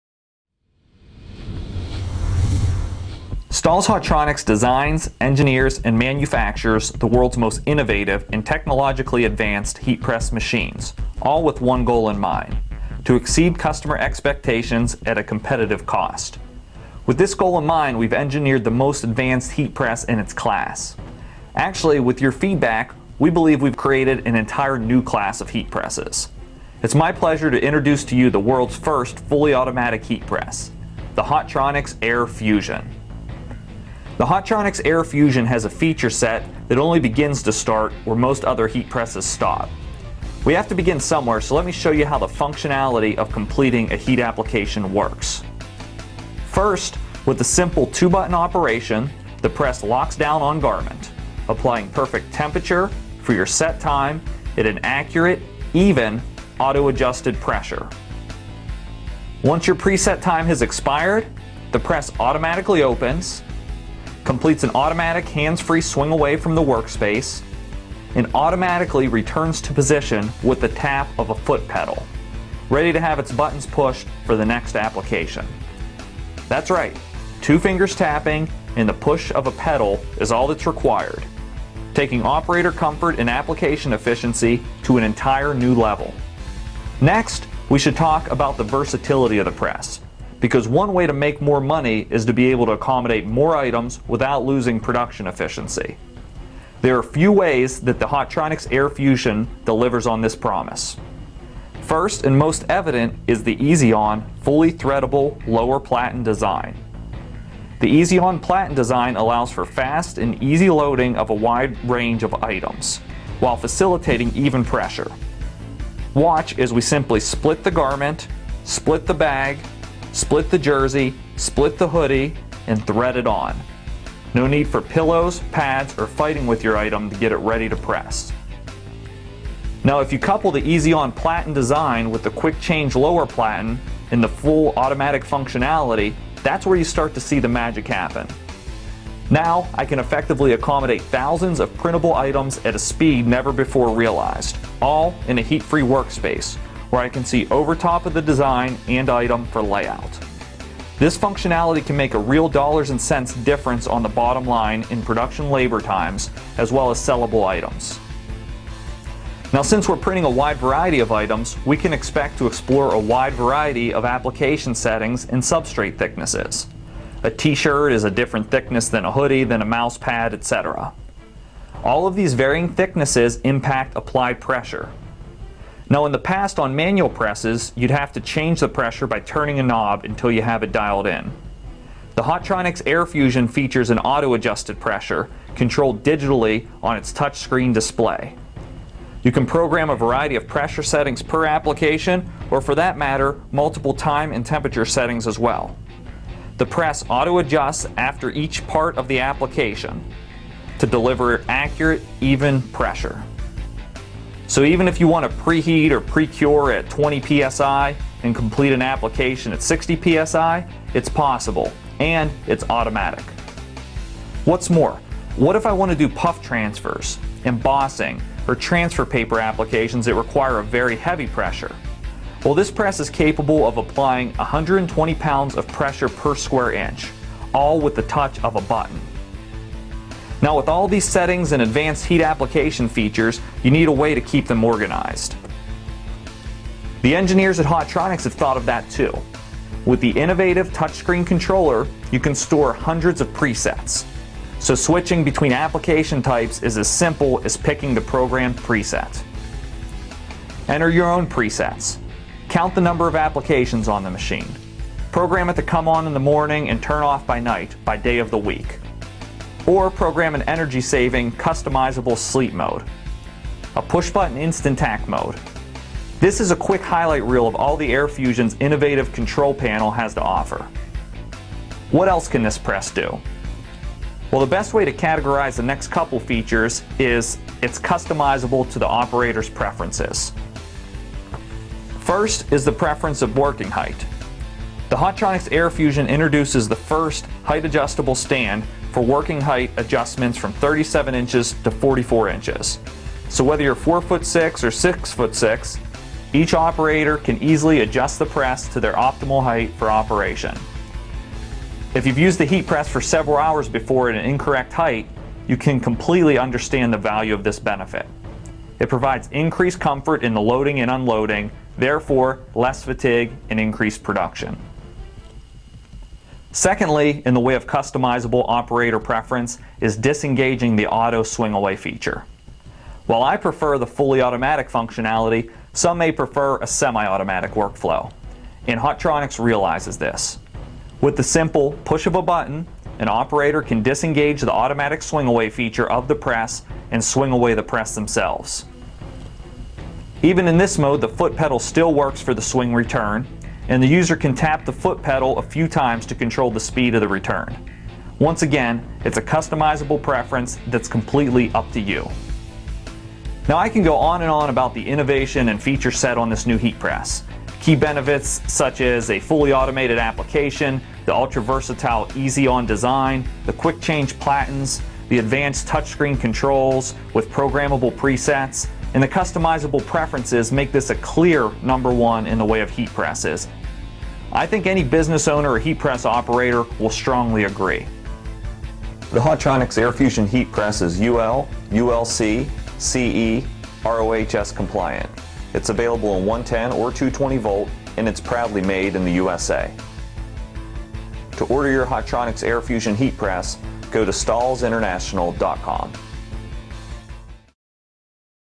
The New Heavy-Duty Air Fusion™ Heat Press -- Designed for High Volume Environments!
The latest Hotronix® heat press was designed for high-volume production, and is perfect for anyone who wants the ultimate in heat printing convenience.

The new Air Fusion heat press sets a new standard for ease of use with its convenient easy-to-load, quick-release lower platen and hands-free auto-swing feature.

Its advanced technology includes an exclusive touch screen controller, data interface port, multiple time and pressure settings, and a pneumatic regulator which allows the press to automatically adjust the pressure for garments placed on the lower platen.